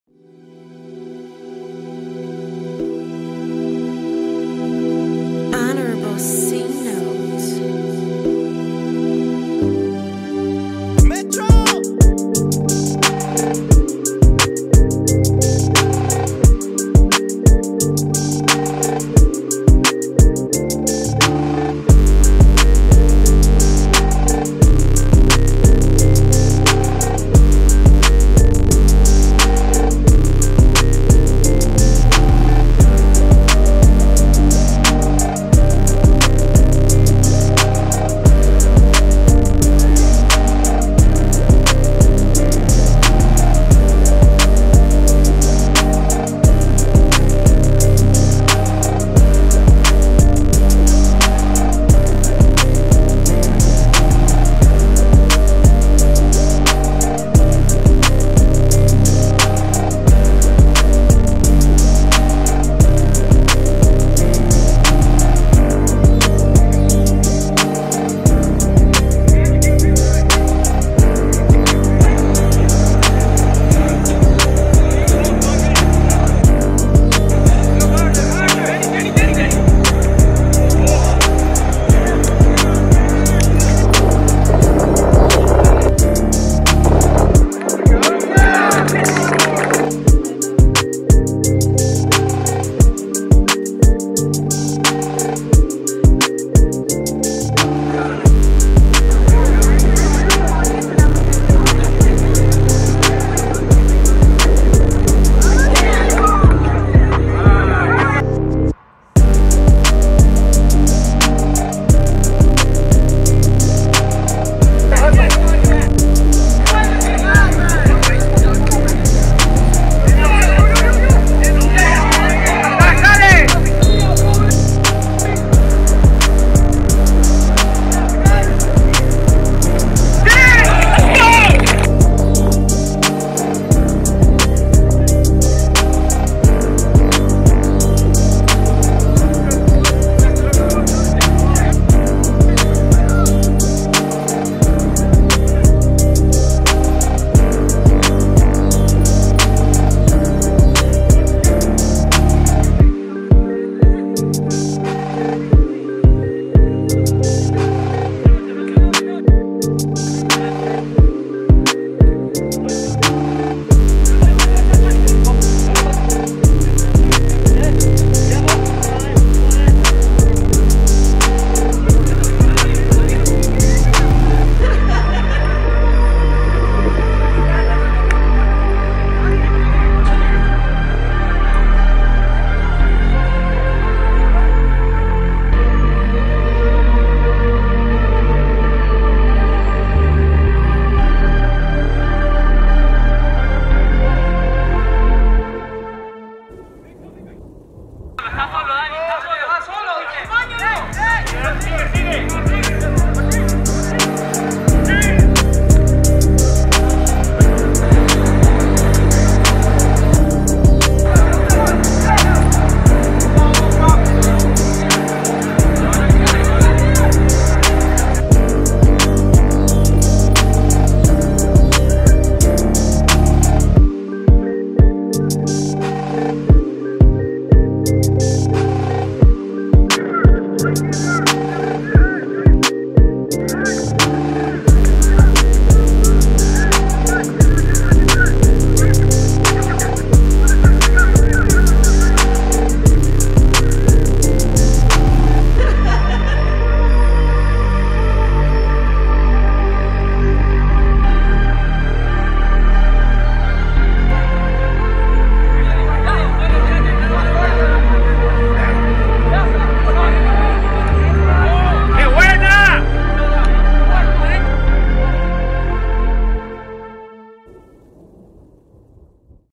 Daniel Ortiz | 2022 - 2023 Season Highlights | Class of 2024
Center Defensive/Holding/Attacking Midfielder for Supreme ECNL u19 and Gulliver Prep. Class of 2024